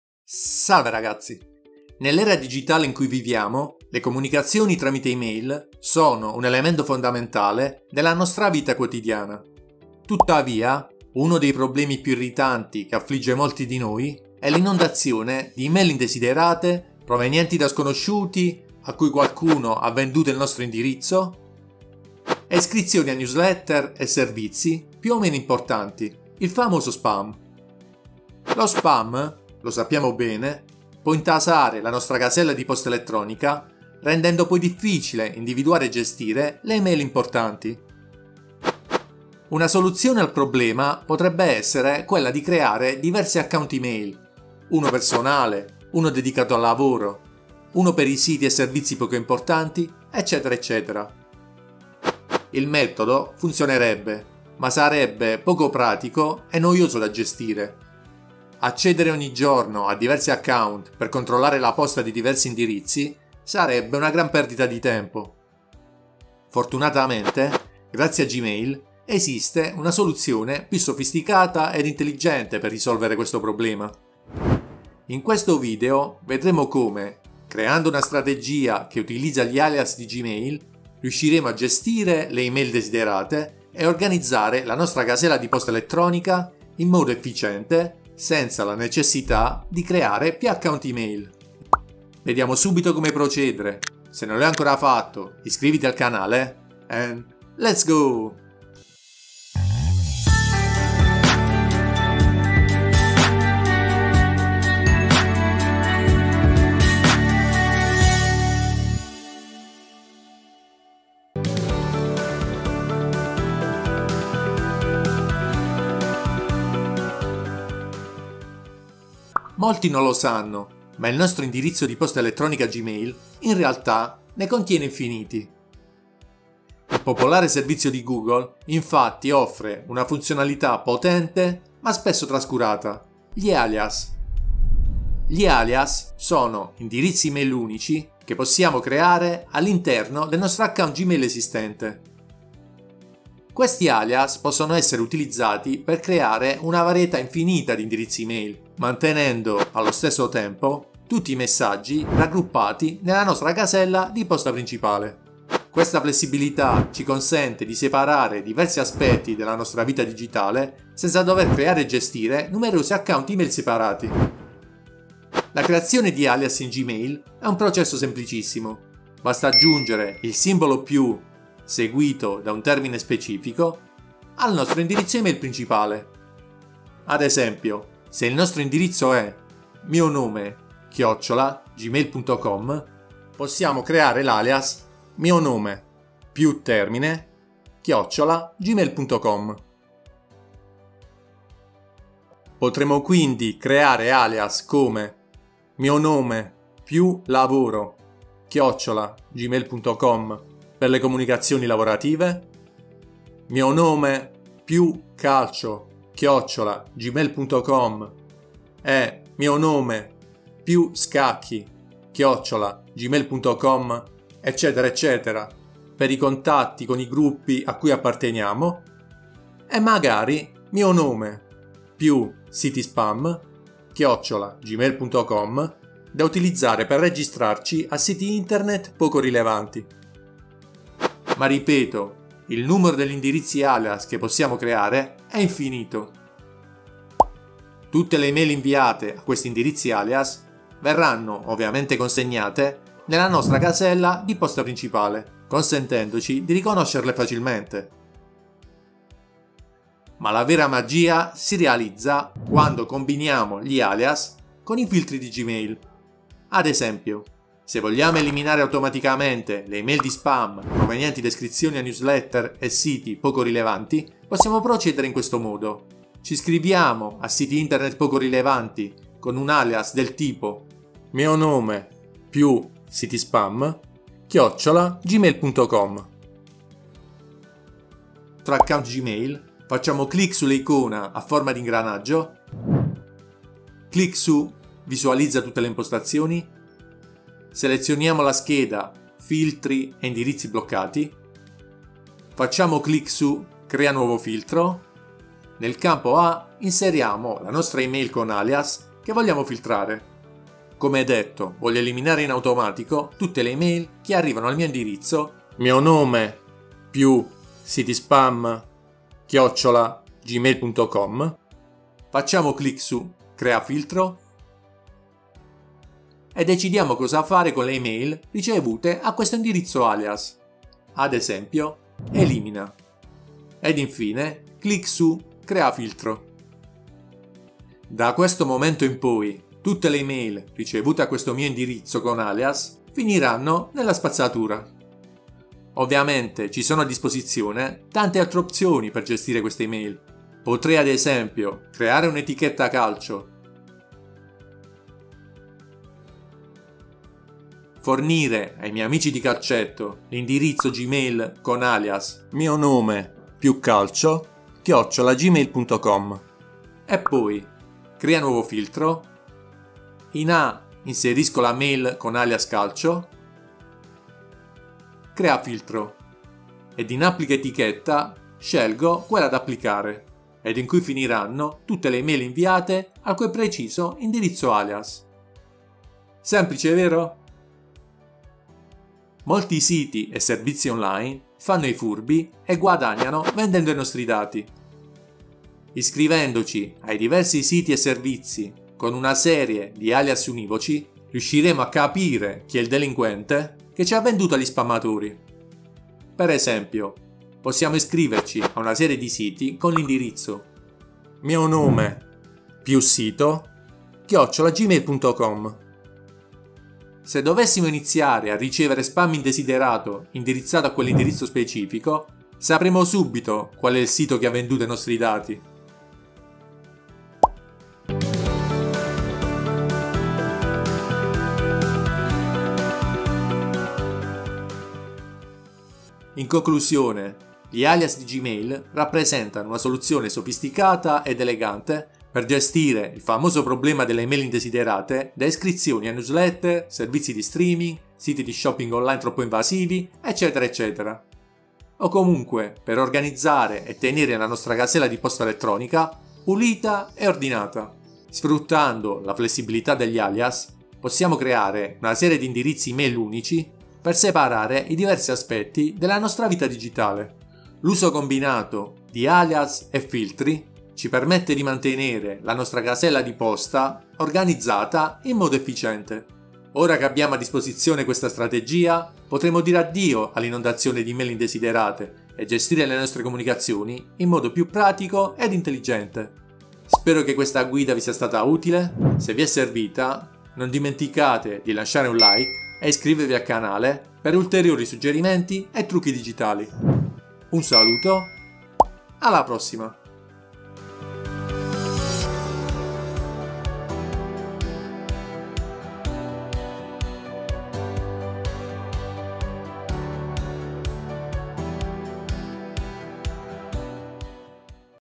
Gmail Alias: organizzare la posta e gestire le Email Indesiderate
Il video mostra come il modo migliore per gestire le email indesiderate e organizzare la  casella di posta elettronica di Google come un vero professionista, tutto grazie agli Alias di Gmail!

"Nell'epoca digitale, le email sono il cuore della nostra comunicazione quotidiana. Tuttavia, il temuto spam può trasformare la nostra casella di posta in un vero caos. Ma ecco una soluzione intelligente offerta da Gmail: gli alias.

🌟 Scopri il Potere degli Alias Gmail 🌟

Gli alias di Gmail sono come magie per la tua casella di posta. Sono indirizzi email speciali collegati al tuo account principale, che semplificano la gestione delle email senza dover creare numerosi account separati.

📨 Gmail: Creazione Semplice degli Alias 📨

Creare alias è un gioco da ragazzi! Basta aggiungere un "+" seguito da una parola chiave al tuo indirizzo principale (esempio: ), e sarai pronto a creare un numero praticamente infinito di alias per scopi diversi.

🚀 Gmail: Vantaggi degli Alias Gmail 🚀

- Gestione Efficiente: Tutte le email vengono consegnate nella tua casella principale, evitando il saltare tra account diversi.
- Separazione delle Comunicazioni: Mantieni distinti lavoro, attività online e iscrizioni con alias specifici.
- Gestione tramite Filtri Gmail: Combina gli alias con i filtri Gmail per liberarti automaticamente dello spam. Risparmierai tempo prezioso!

🔒 Gmail: Sicurezza e Best Practices 🔒

Ricorda di proteggere la tua privacy con password robuste e l'autenticazione a due fattori. Non condividere gli alias con persone non autorizzate.

🌟 Gmail:  Esempi Pratici e Testimonianze 🌟

Molti hanno beneficiato degli alias di Gmail per scoprire chi vende i loro dati agli spammatori. Questo sistema comunque è solo uno dei tanti benefici che gli alias possono portare nella nostra vita digitale.

🎉 Gmail:  Conclusioni 🎉

Gli alias di Gmail sono la tua arma segreta per eliminare lo spam e organizzare la tua casella di posta. Rendi la tua esperienza email più intelligente e ordinata oggi stesso!

▬▬▬▬  VIDEO CORRELATO  ▬▬▬▬

▬▬▬▬  ARTICOLI  ▬▬▬▬

▬▬▬▬  SEGUIMI QUI  ▬▬▬▬

▬▬▬▬  RICHIESTA VIDEO ▬▬▬▬

🙏 - Metti un like a tutti i video!
GRAZIE!!! 🙌🙌🙌